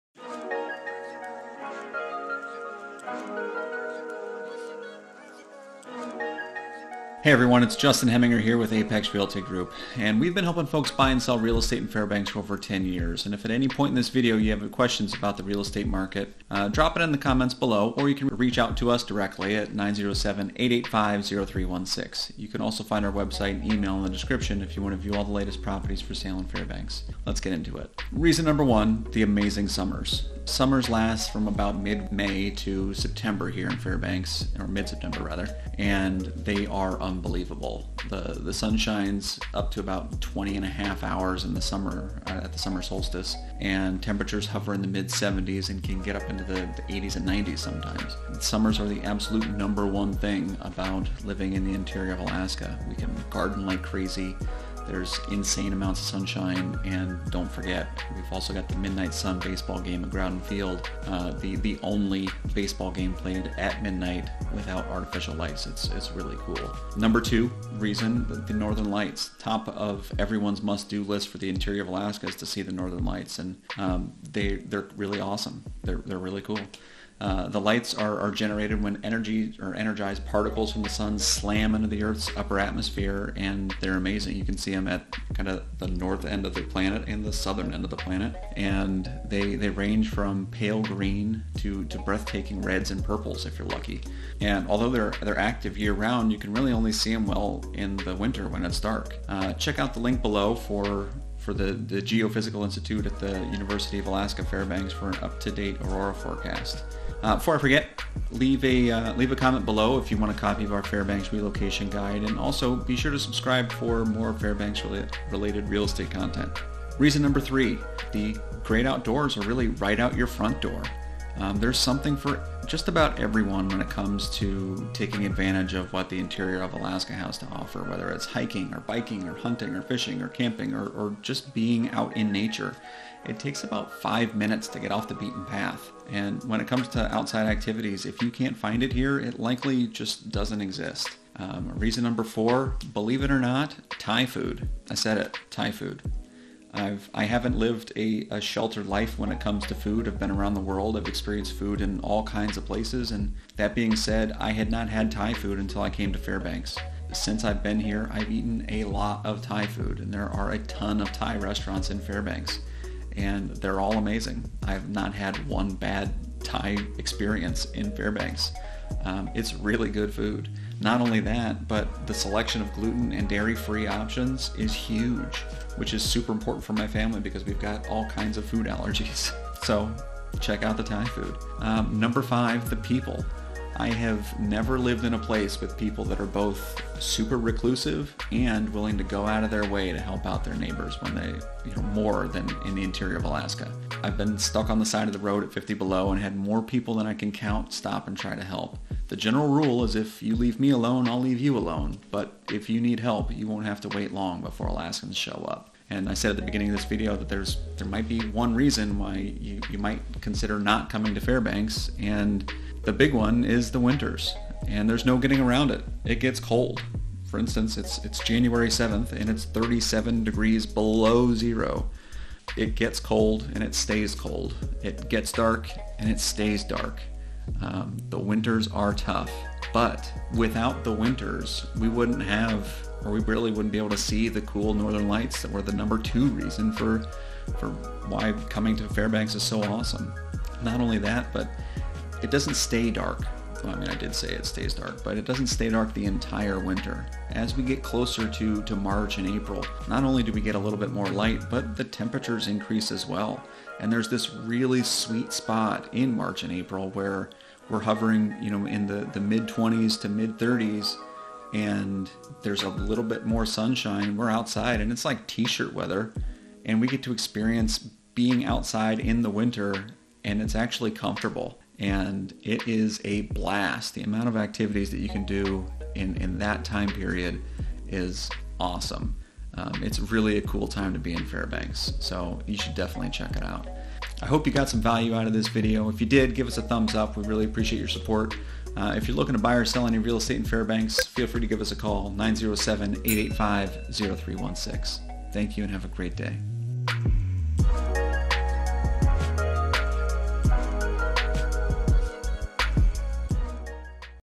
Top 5 Reasons We live in Alaska!
In this video we break down the top 5 reasons why we moved to Fairbanks. We love it here, and we think you will too. If you’re considering making the move, we’d be more than happy to chat and answer any questions you have, just drop a comment below or reach out to us directly at !